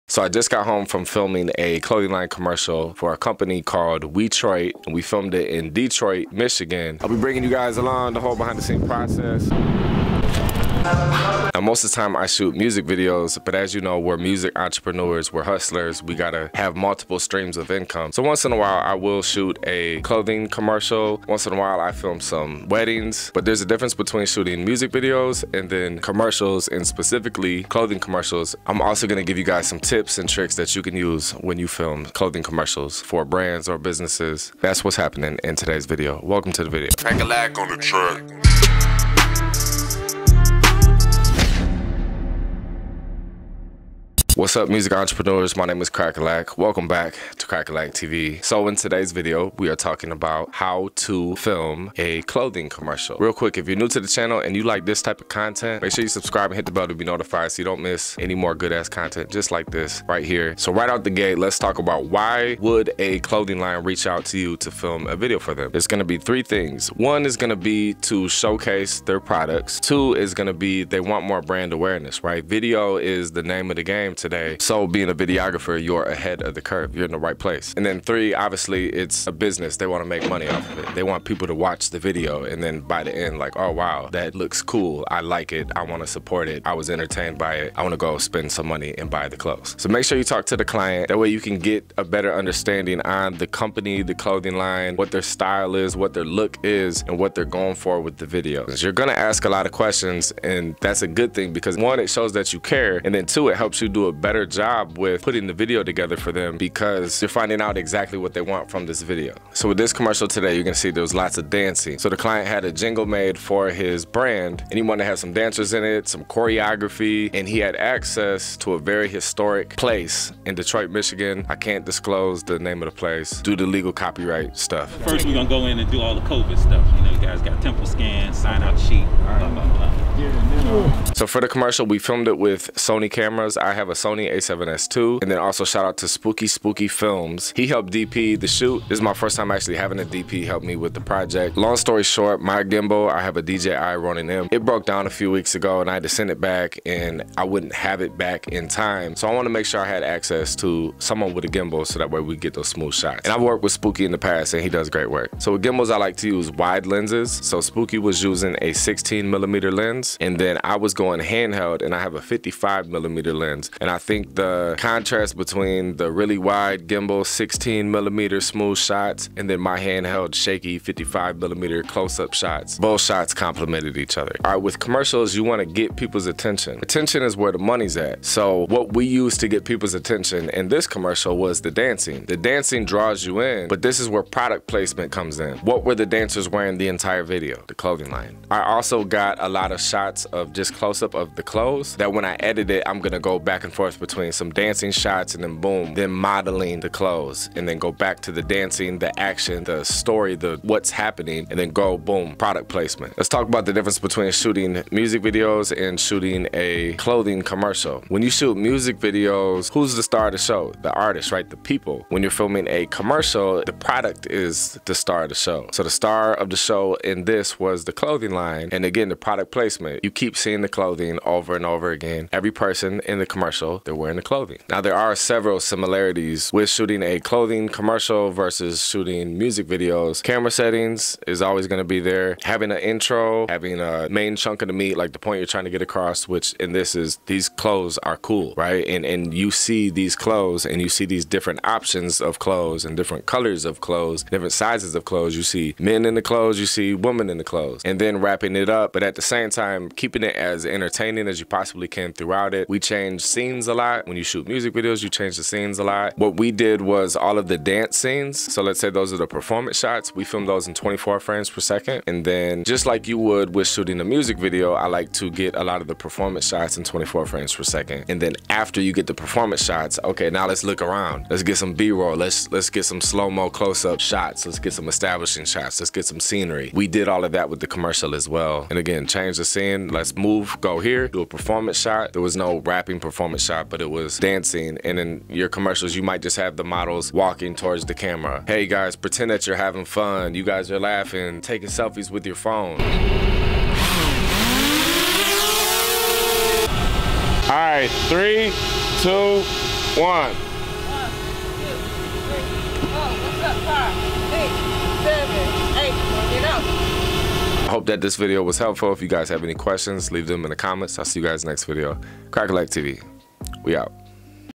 How To Film a Clothing Commercial
Want to learn "How To Make a Living Shooting Music Videos"?

In this video Cracka Lack walks you through how to film a clothing commercial. Also see behind the scenes of him filming a clothing commercial.

Video Edited by Ayoub bd. Filmed by Malik Brand.